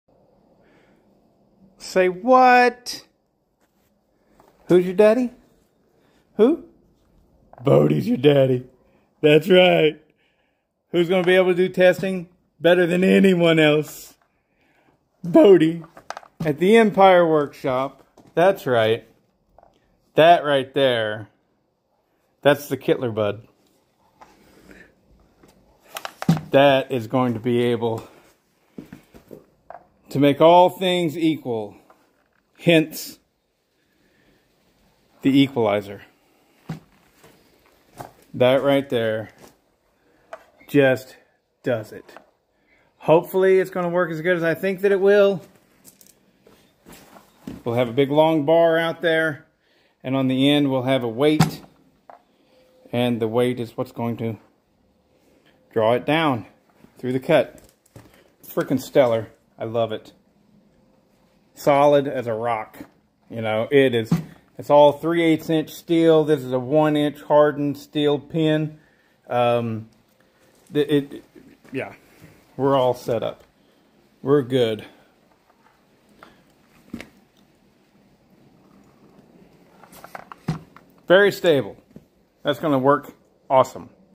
Look out and SHOUT! This rig will take testing to another level.
A custom fabricated chainsaw jig that will allow equal power comparisons.  To remove any and all operator errors.